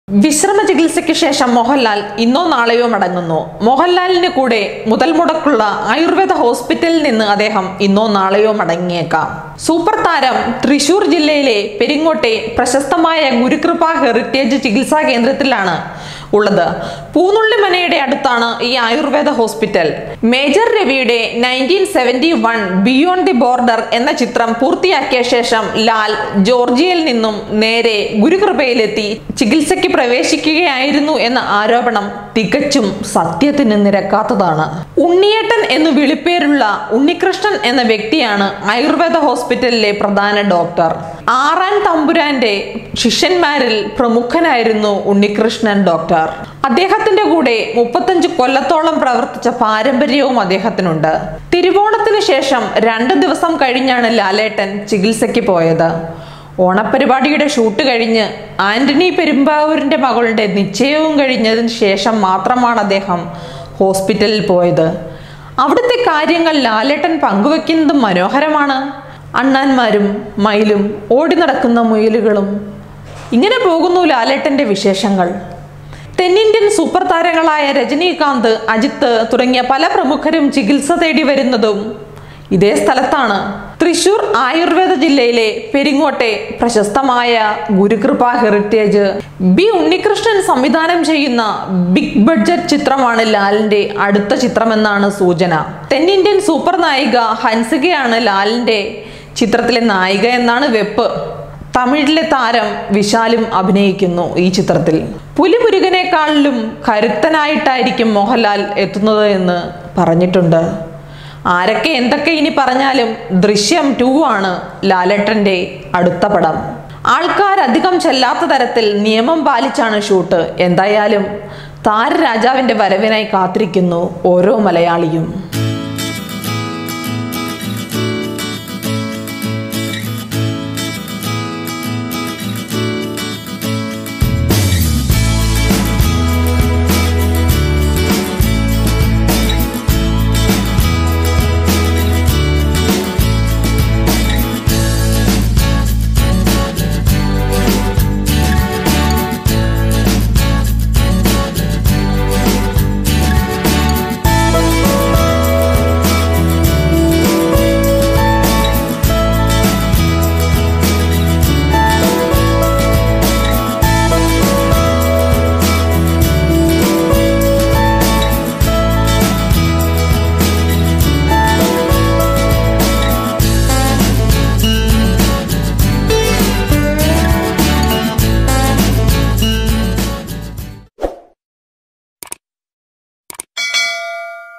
ലാലേട്ടന്റെ ആ മടക്കത്തിൽ | Mohanlal
മോഹൻലാലിന് കൂടെ മുതൽ മുടക്കു ഉള്ള ആയുർവേദ ഹോസ്പിറ്റലിൽ നിന്ന് അദ്ദേഹം ഇന്നോ  നാളെയോ മടങ്ങിയേക്കാം